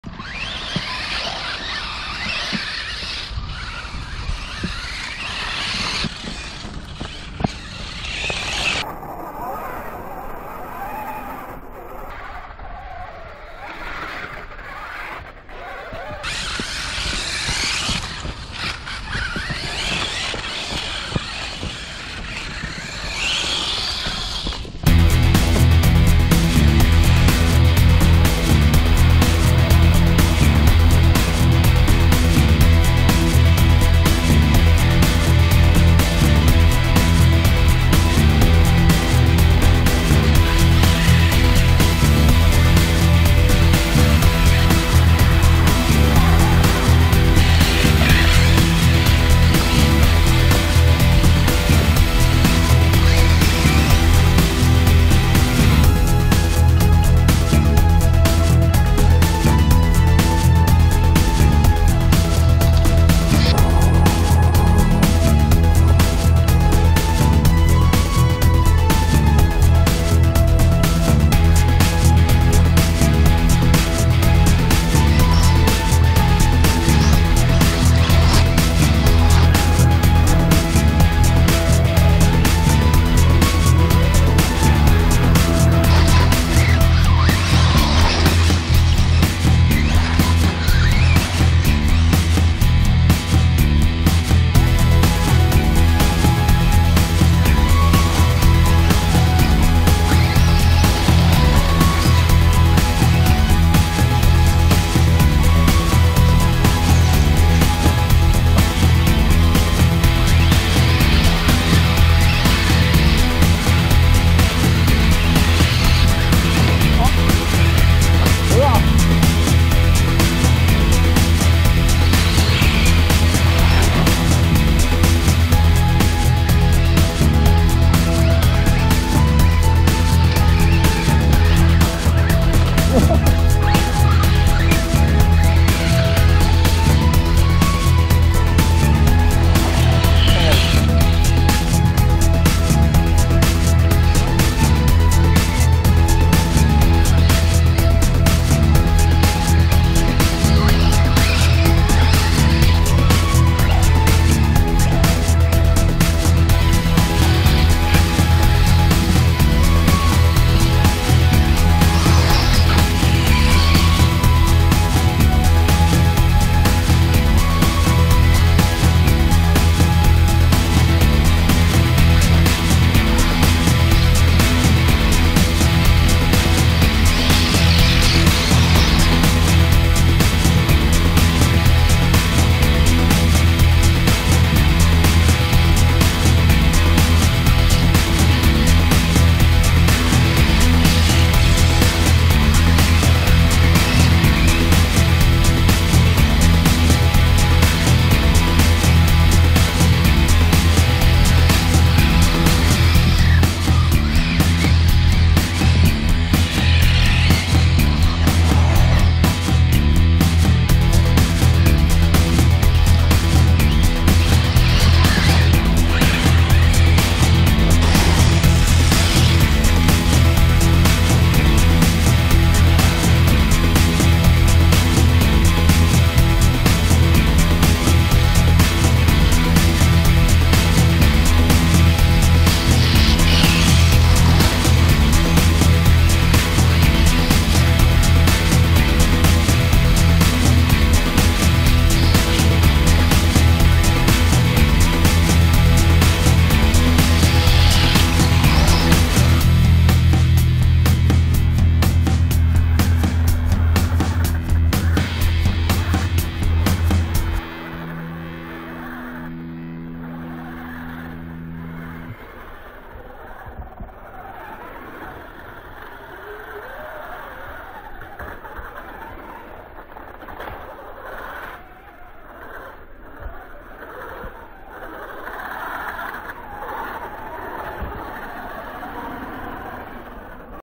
Losi Ford Raptor Baja Rey and Tenacity TT Pro / 1/10 Short Course Truck Race
Losi
1/10 King Shocks Ford Raptor Baja Rey
Losi
1/10 4WS Short course Truck Tenacity TT Pro

 Short course Truck  on a backyard Rc car Track

offroad Racing
Truck vs Truck